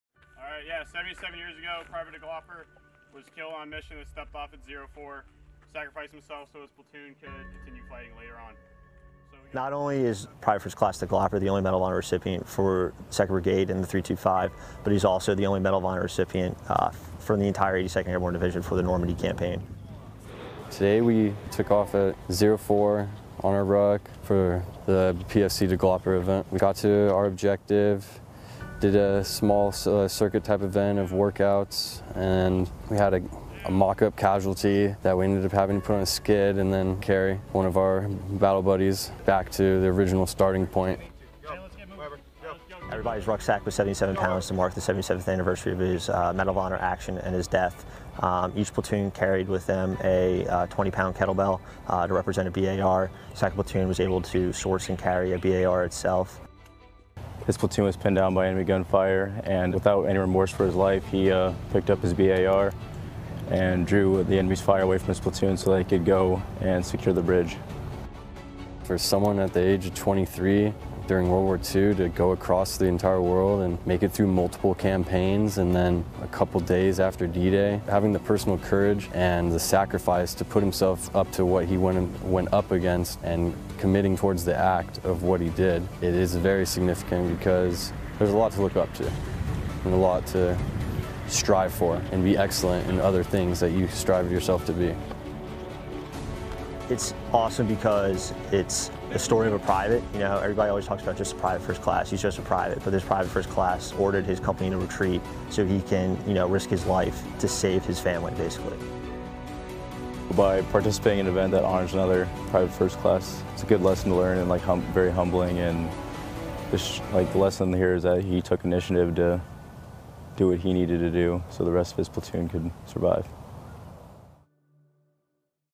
U.S. Army Paratroopers from Charlie Company 77 years after the heroic and sacrificial actions of PFC
PFC DeGlopper Hero WOD
UNITED STATES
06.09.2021

Seventy-seven years after the heroic and sacrificial actions of PFC Charles DeGlopper, U.S. Army Paratroopers from Charlie Company, 2nd Battalion, 325th Airborne Infantry Regiment, 2nd Brigade Combat Team, 82nd Airborne Division, conducted a Hero Workout of the Day in his honor. PFC DeGlopper received the Medal of Honor for sacrificing his life to save his company during the Normandy Campaign on 09 June 1944.

LowerThirds:
1LT Adam Arata
1st Battalion 325th, 2BCT

PFC Justin Gubany
1st Battalion 325th, 2BCT

PFC Nick Ethier
1st Battalion 325th, 2BCT

Film Credits: Video by Sgt. Catessa Palone
49th Public Affairs Detachment